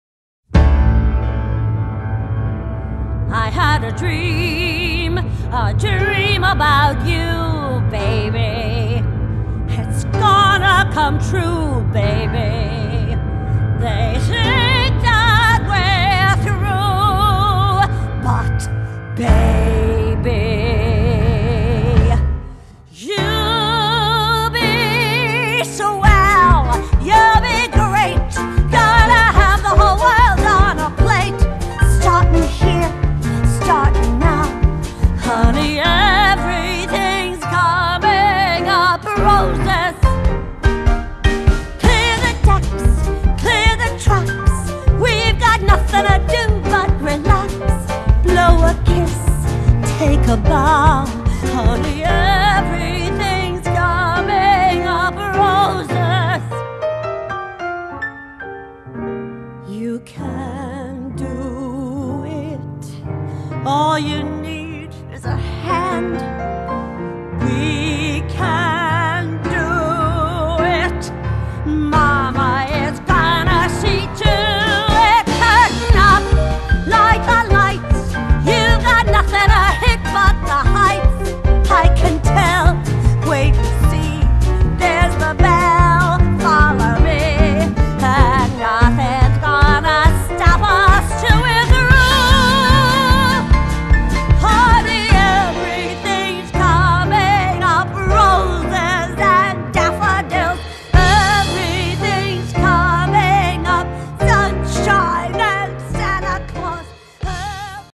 Bald Stageart Rundgang
Ein kurze blick in das Herz von Stageart Musical School Hamburg